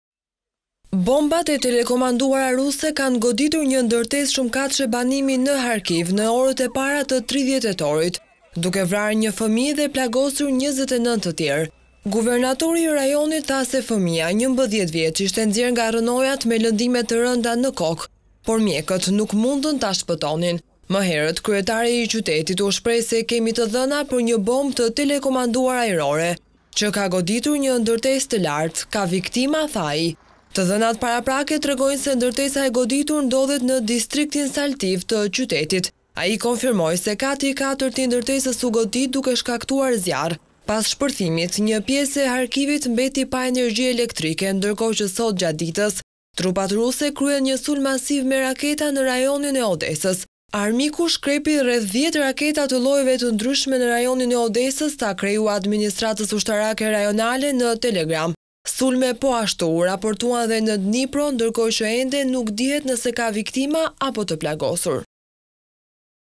Top Channel/ Humb jetën një fëmijë nga bombardimet në Kharkiv, plagosen 29 të tjerë
Bombat e telekomanduara ruse kanë goditur një ndërtesë shumëkatëshe banimi në Harkiv në orët e vona të 30 tetorit, duke e vrarë një fëmijë dhe duke i plagosur 29 njerëz të tjerë, thanë zyrtarët.
Guvernatori i rajonit të Kharkivit, Oleh Synyehubov, tha se fëmija, 11 vjeç, ishte nxjerrë nga rrënojat me lëndime të rënda në kokë, por mjekët nuk ishin në gjendje ta shpëtonin.
Më herët, kryetari i qytetit të Kharkivit, Ihor Terekhov, u shpreh se “kemi të dhëna për një bombë të telekomanduar ajrore që ka goditur një ndërtesë të lartë. Ka viktima”.
Të dhënat paraprake tregojnë se ndërtesa e goditur ndodhet në distriktin Saltiv të qytetit. Ai konfirmoi se kati i katërt i ndërtesës u godit, duke shkaktuar zjarr. Pas shpërthimit, një pjesë e Kharkivit mbeti pa energji elektrike, ndërkohë që sot gjatë ditës trupat ruse kryen një sulm masiv me raketa në rajonin e Odesës.
“Armiku shkrepi rreth 10 raketa të llojeve të ndryshme në rajonin e Odesës”, tha kreu i administratws ushtarake rajonale në Telegram.
Sulme u raportuan po ashtu ne Dnipro.

Pamje: AFP